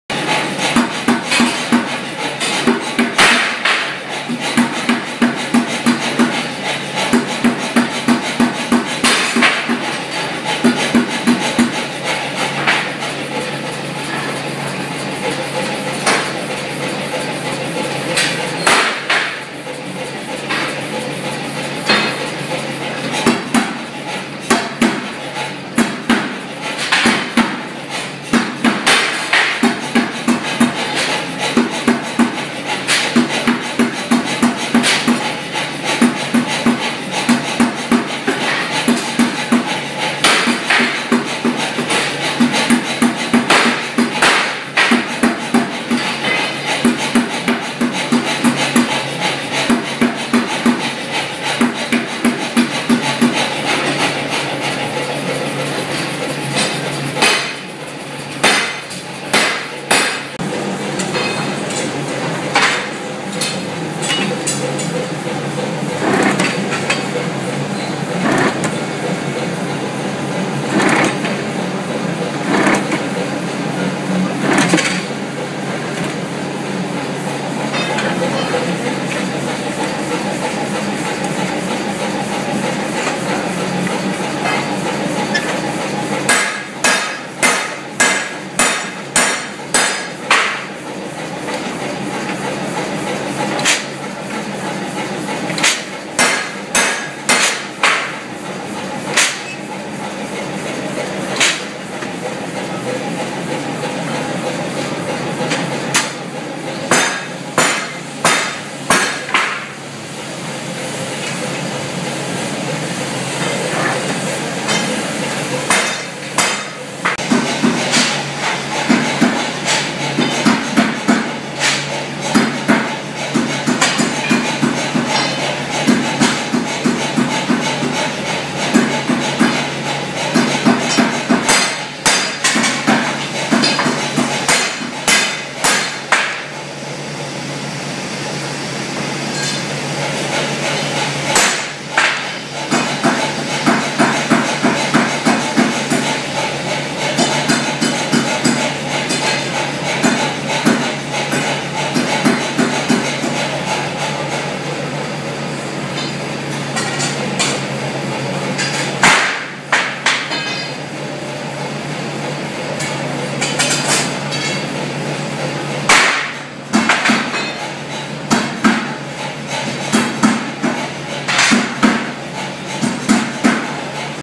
The Ironart team making 'Bath' style finials
This short film shows three guys in the Ironart workshop, working together to batch out a large quantity of 'Bath' style finials for a new development on the edge of town. Dominic West heats the bars in the gas forge, then tapers the ends of each bar using the power hammer, he uses dividers to check the length of each taper. James Cuthbertson then forms the profile of each finial using the hydraulic press with a forming tool in it, then he works on the fly press putting chisel lines into each one. Finally, after they have been linished to remove the scale and ensure a square point, Ted Powles visually checks each one for straightness, any last adjustments are made using a copper hammer on a straightening block.